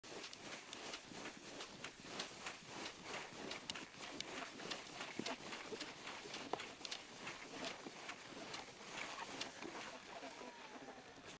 No edge swivel Wedeln at 2 km per hour!
no edge swivel turns on super flat terrain are much harder than you think! There is no edge set for rebound and synchronising the feet is really really hard.

@projectedproductions - how I improve my skiing
@origplus - The skis used in this are the MOD 6X 175cm 16m radius ski
@carvesport_com - are the ski wear I use in this video